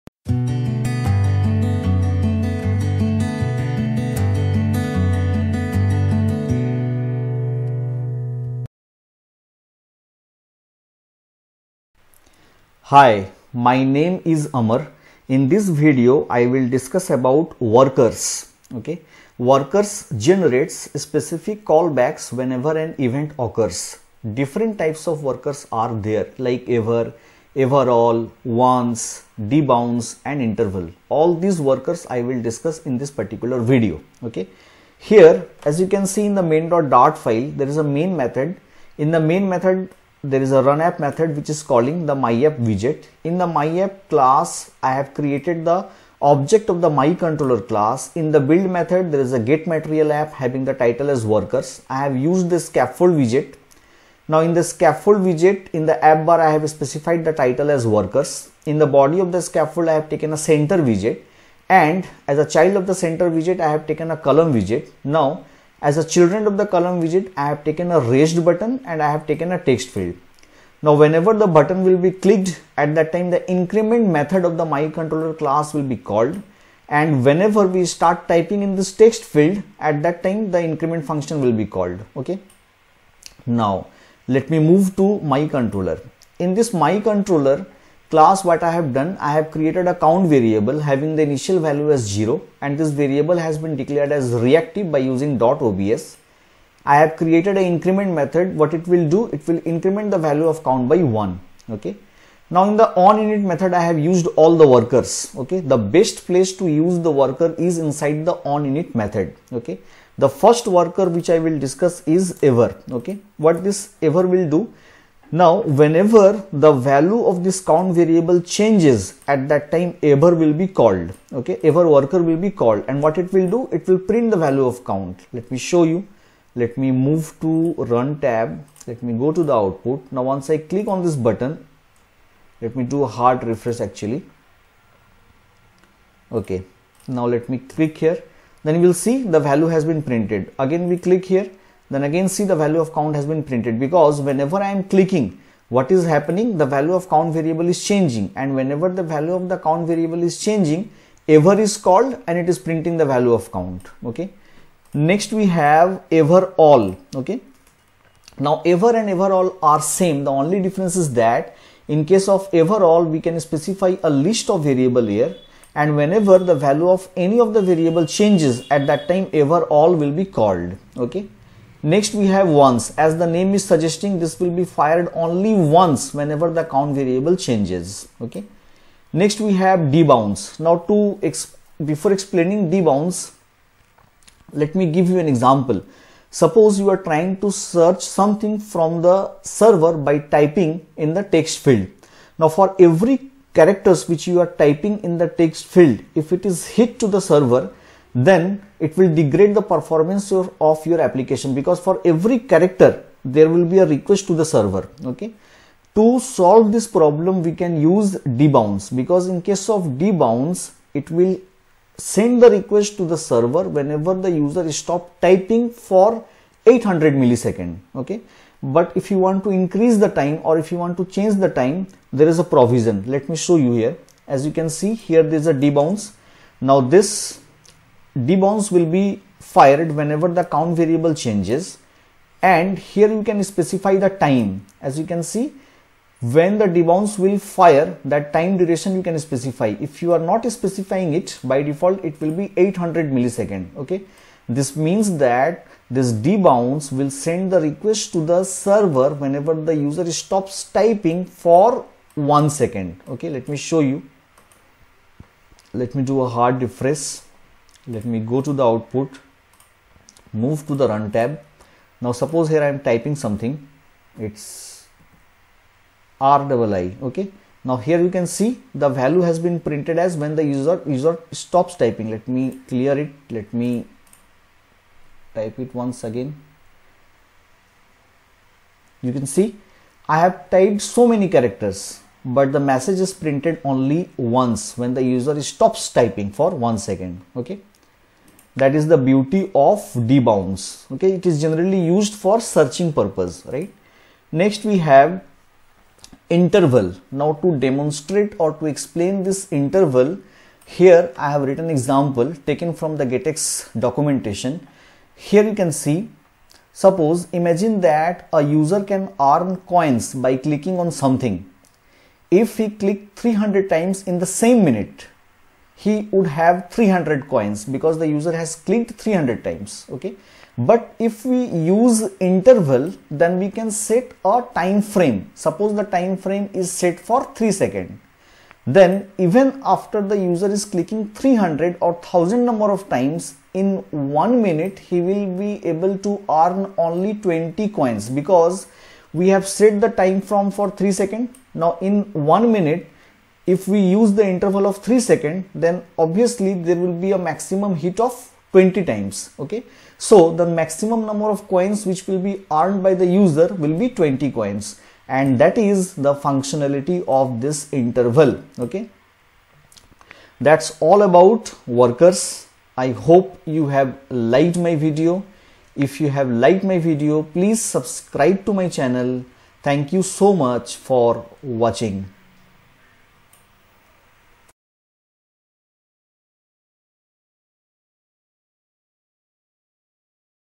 #14 || Flutter GetX Tutorial || Understanding GetX Workers
In this video I have discussed about workers in getx